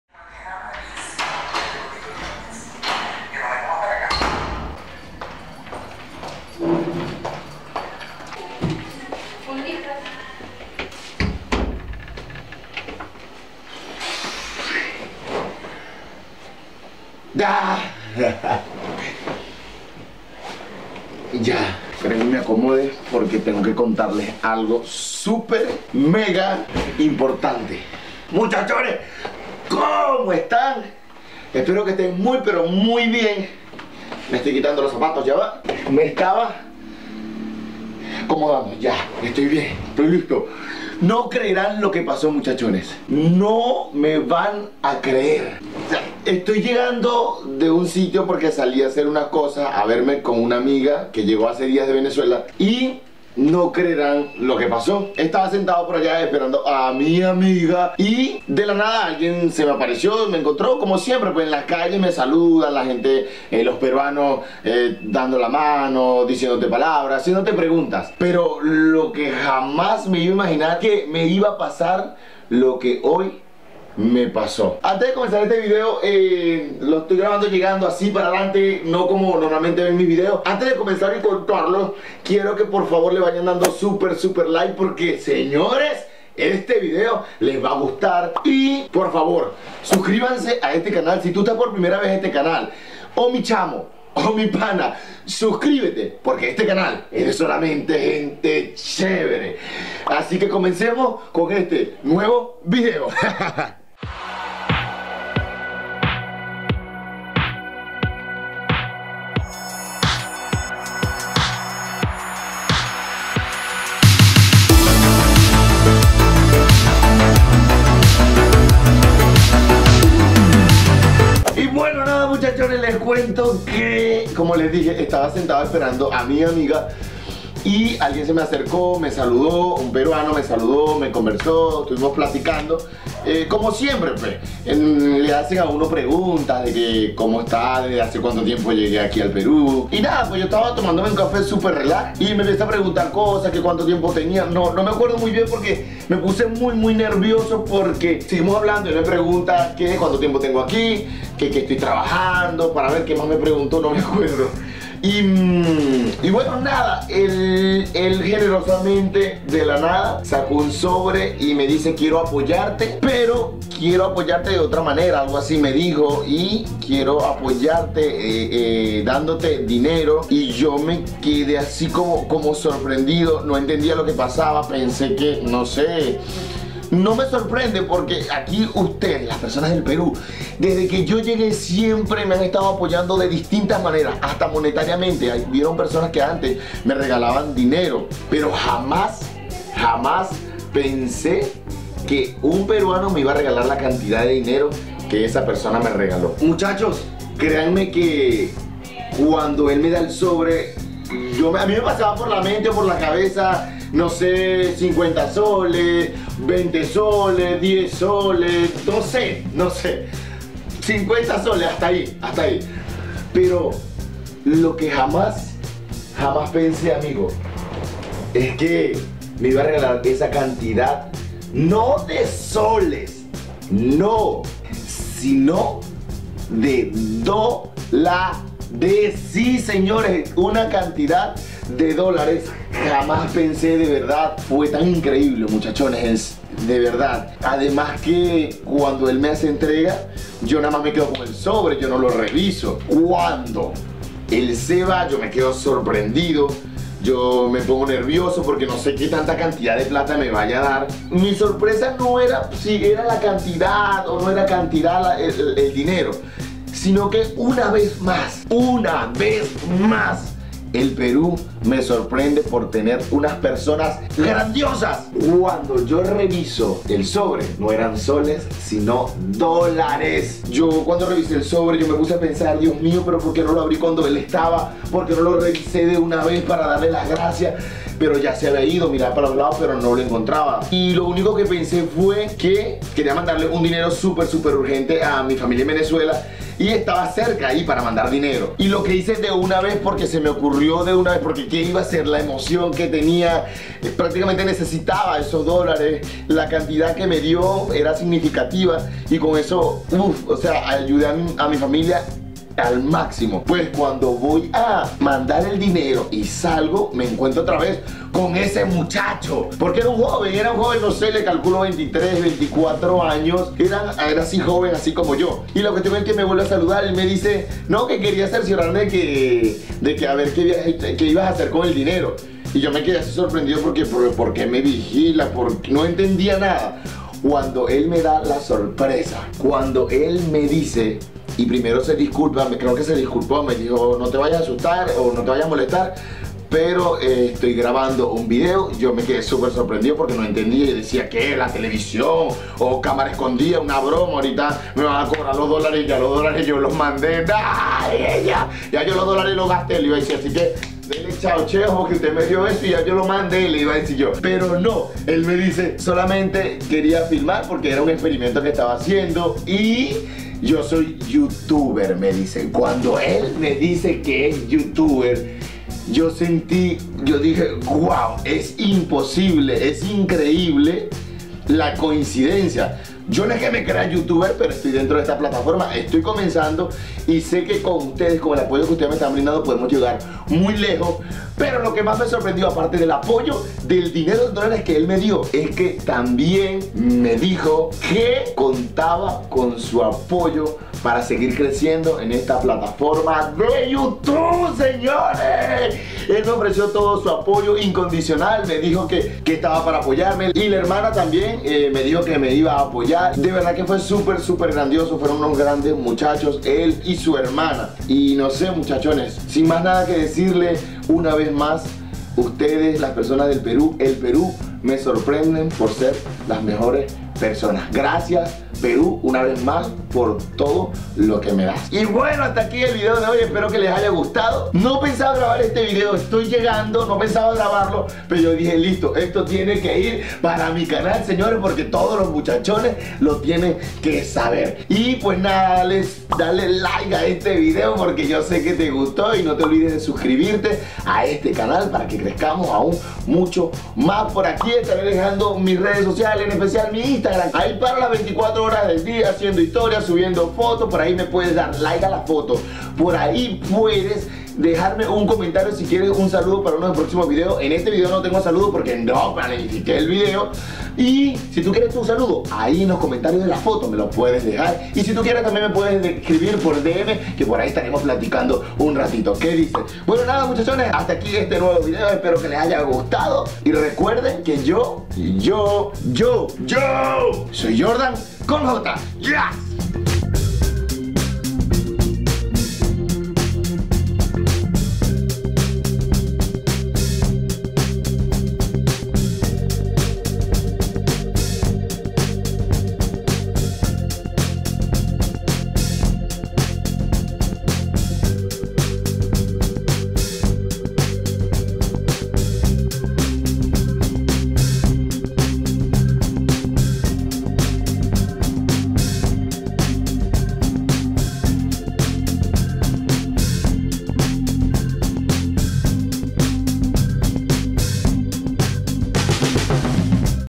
DI EQUISDE ME REGALA ALGO INCREÍBLE | VENEZOLANO EN PERÚ
Muchachoneeees les quiero compartir lo que me paso estando en una plaza, porque me regalaron algo que jamas imagine que una persona me regalaría, el Perú me sigue sorprendiendo...

Redes de mi Editor:
Yorman Hurtado